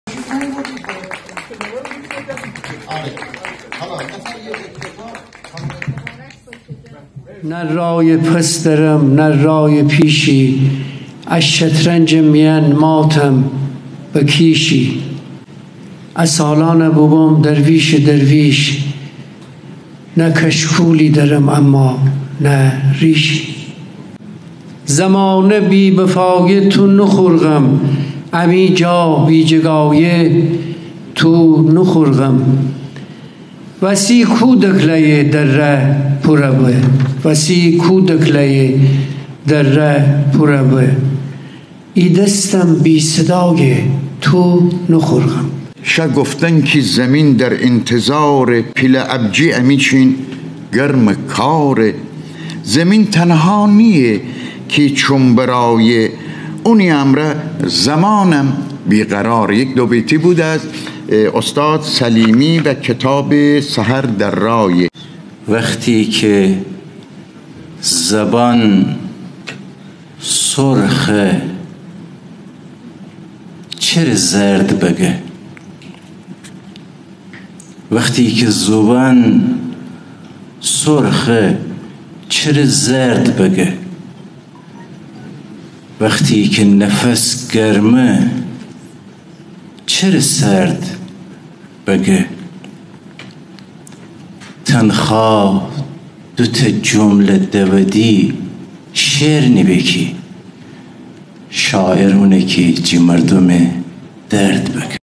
هادی سلیمی دافساری - رونمایی کتاب شعر "سحر در رایه"
رونمایی کتاب شعر "سحر در رایه"
✍️ سروده: هادی سلیمی دافساری
باحضور تنی چند ازشاعران و هنرمندان گیلانی به مدیریت جناب مسعود ایمانی و شعرخوانی علیرضا صدیق دوبخشری
دوشنبه (۳۰خرداد۱۴۰۱) خمام
🎥 کلیپ: پوریا همتی